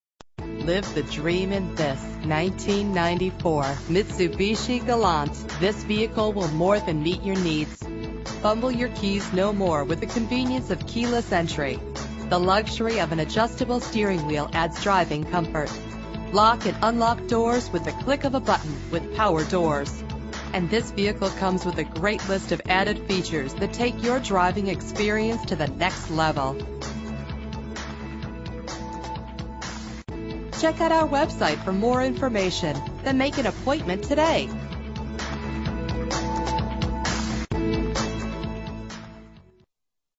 1994 Mitsubishi Galant in Buford, GA
3505 Buford Drive (Ga Hwy 20)
Buford, GA 30519
1994 Mitsubishi Galant

Mileage:69282